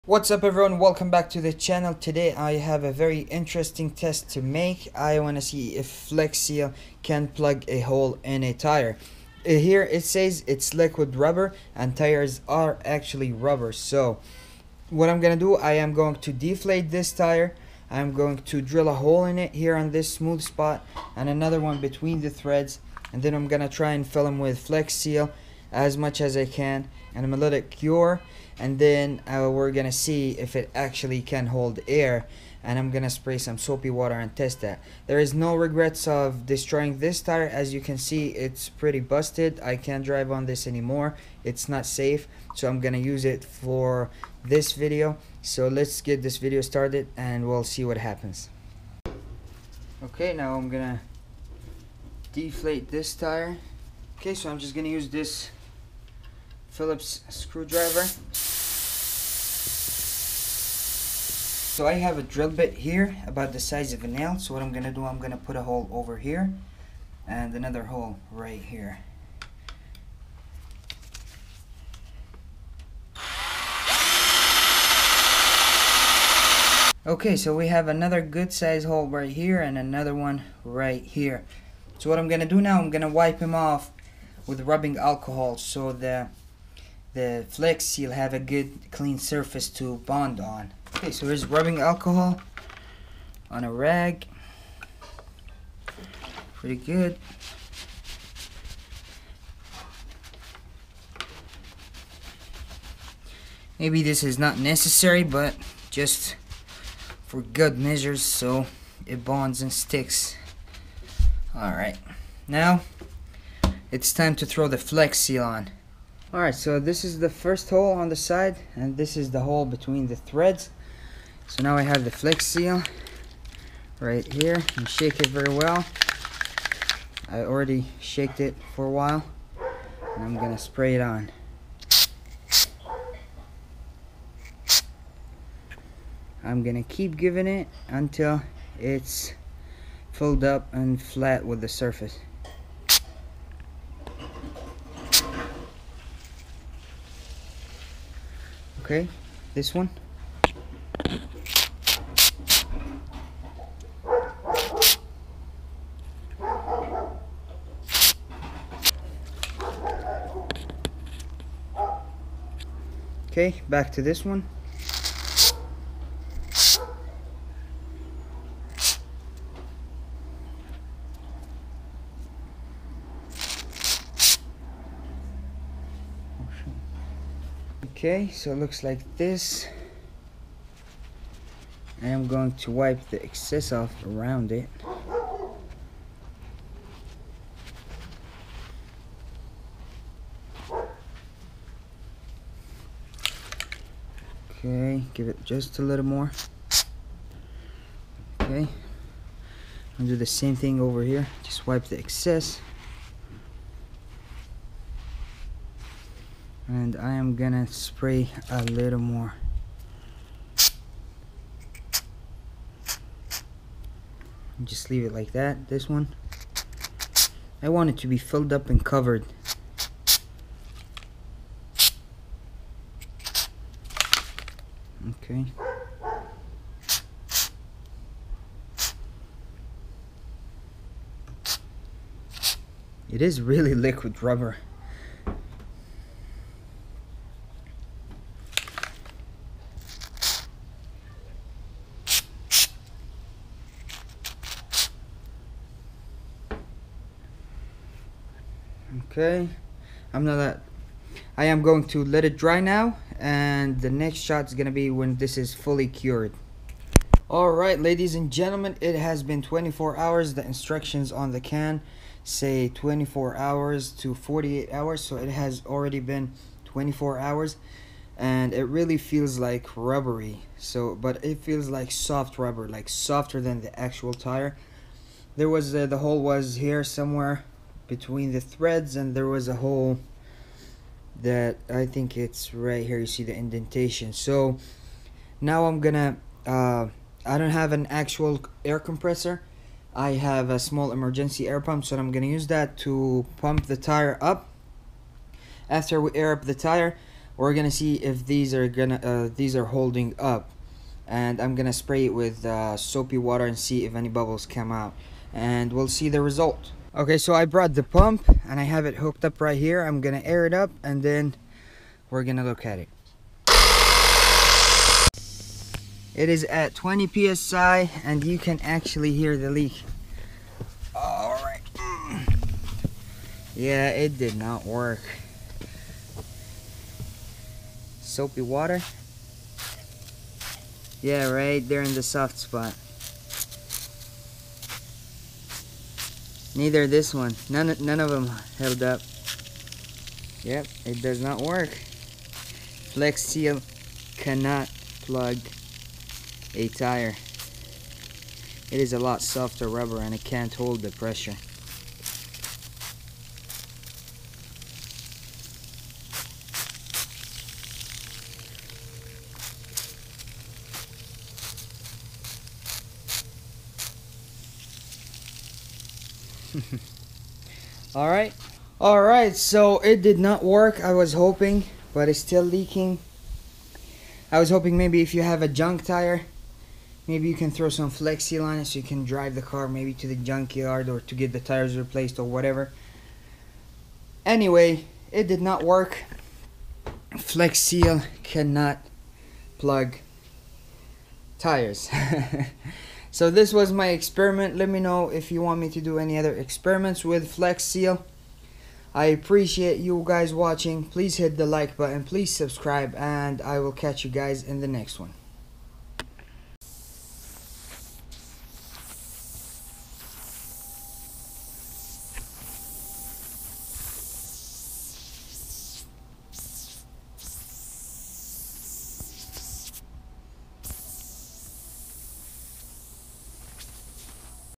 FLEX SEAL: Can it Fix FLAT Tire? Let’s Find Out!
Can Flex Seal fix a flat tire, trying FLEX SEAL as a Fix a flat, I will drill a hole and fill it with Flex Seal.